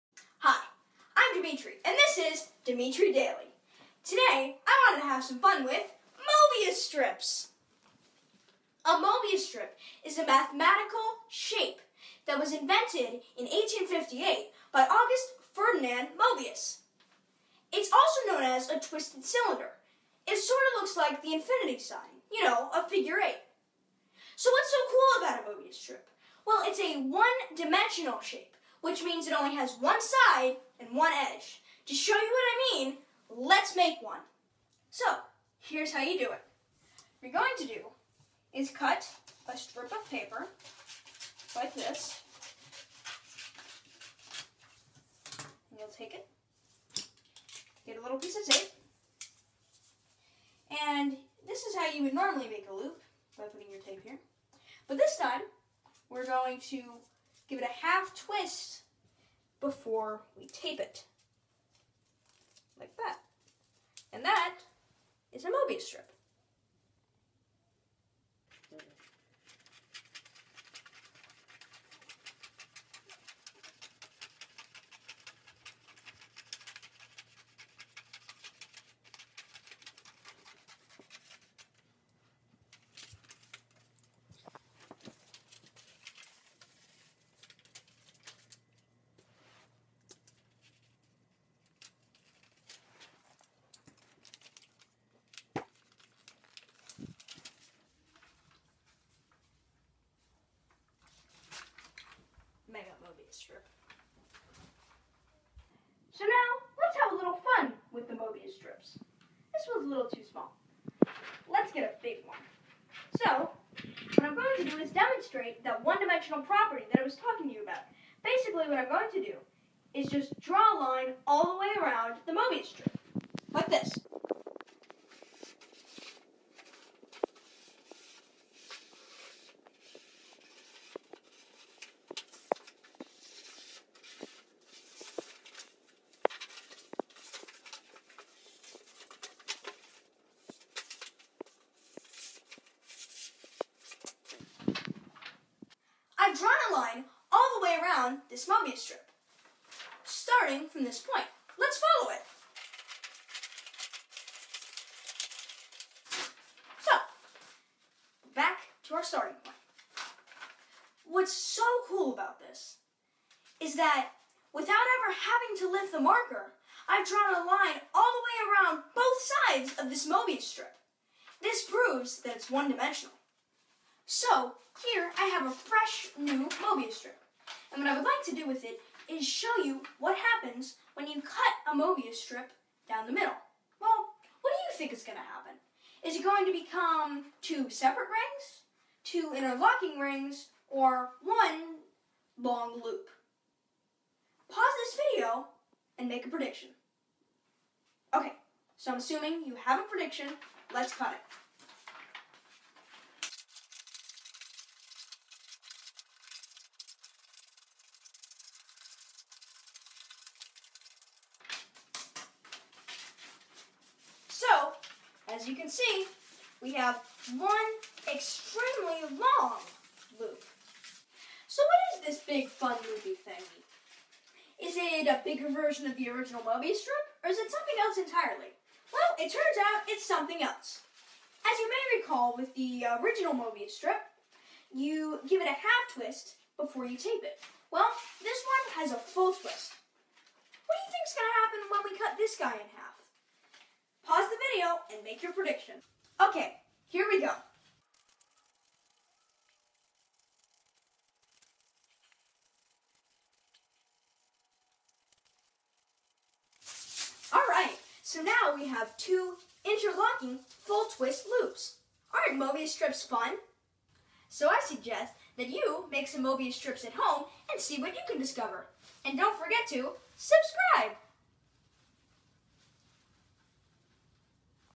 Mobiüs Strips
In this video I explain how to make Mobiüs strips.